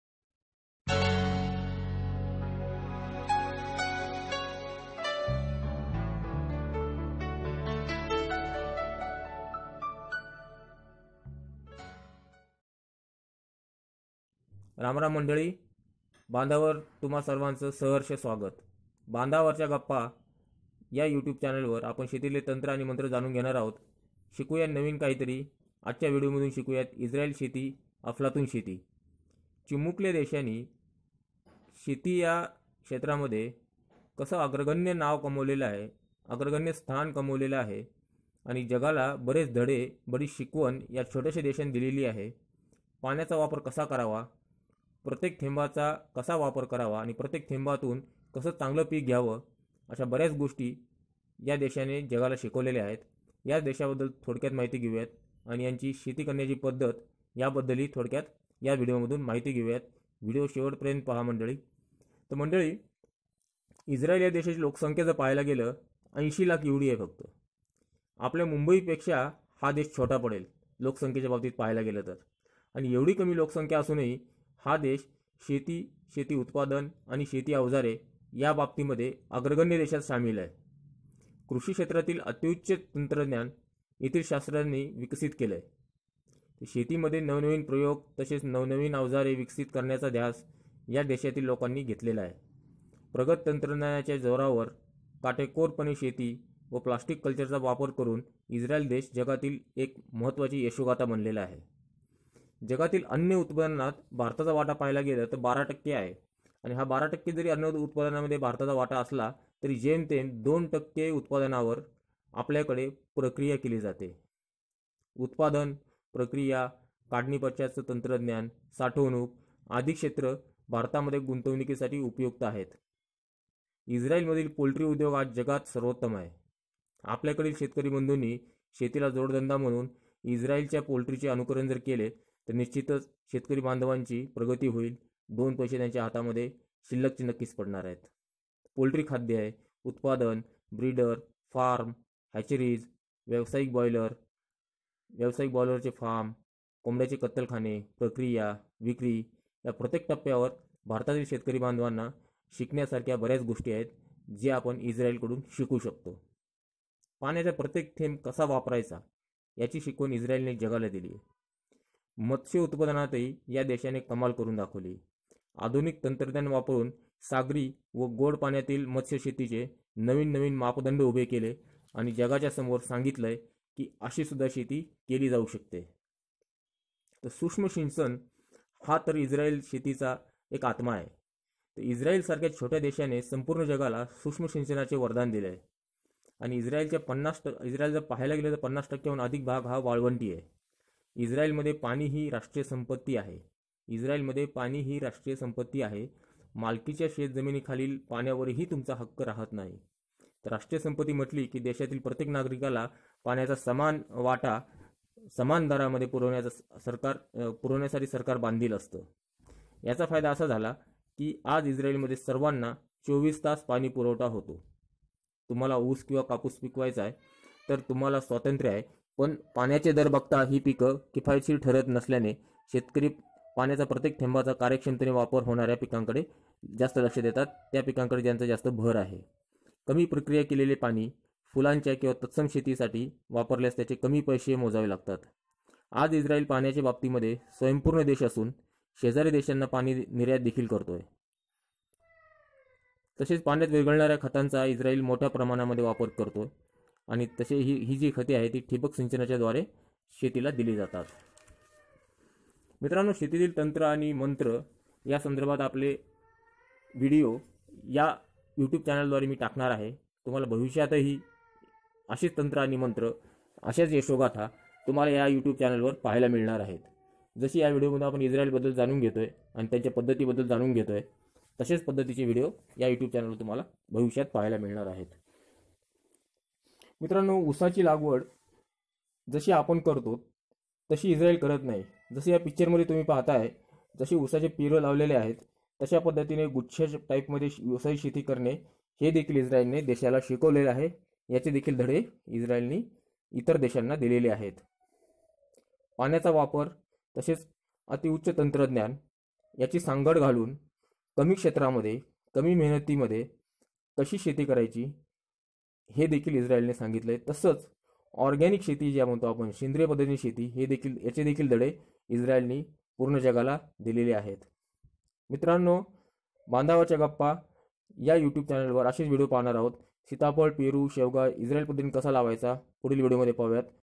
इस्त्राईल शेती | इस्त्राईल शेतीचे तंत्र-मंत्र | इस्त्राईल शेतीचे तंत्रज्ञान
रामराम मंडळी,

इस्त्राईल देश शेती मध्ये अग्रगण्य देशात सामील आहे.
इस्त्राईल पद्धतीने आपल्याकडील पिके कशी लावावीत व भरघोस उत्पन्न कसे घ्यावे या संदर्भातील व्हिडीओ " बांधावरच्या गप्पा" या youtube channel वर आपण भविष्यात पाहणार आहोत.

नमस्कार.

बांधावरच्या गप्पा.